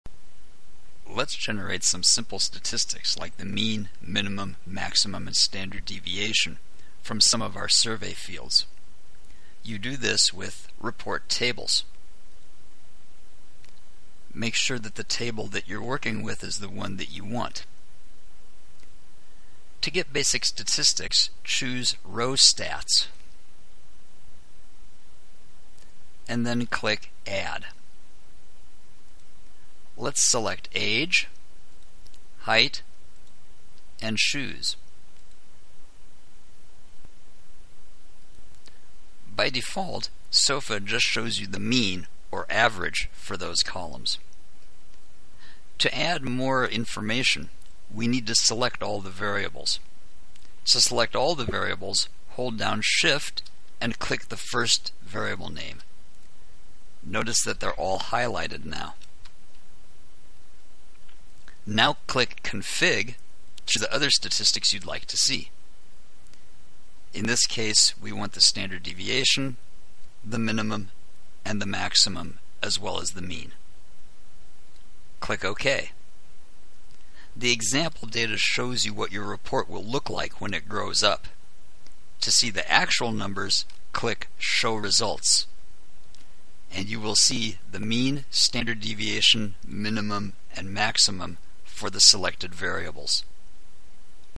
Calculating Basic Statistics with SOFA
Calculating mean, standard deviation, minimum, and maximum with SOFA